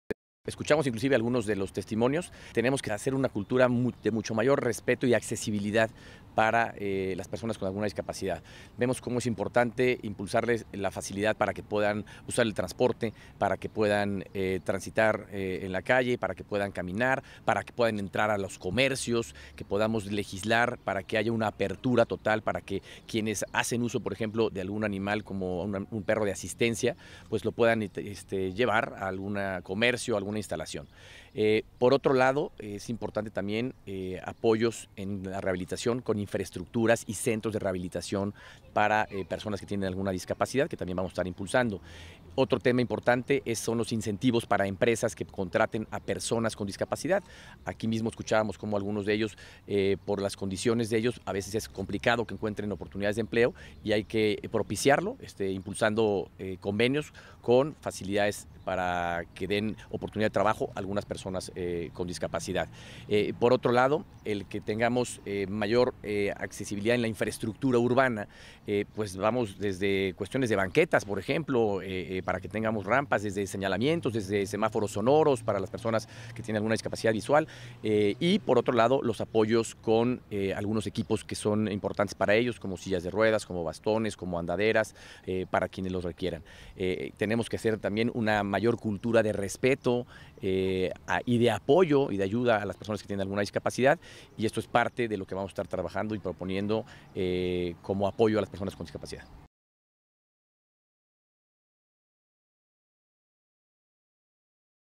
Encuentro de Alfredo del Mazo con discapacitados mexiquenses
El candidato a la gubernatura del Estado de México por la coalición PRI-PVEM-PANAL-PES, Alfredo del Mazo Maza, se reunió con discapacitados mexiquenses con quienes se comprometió a mejorar la infraestructura urbana para que sea amable con toda la población.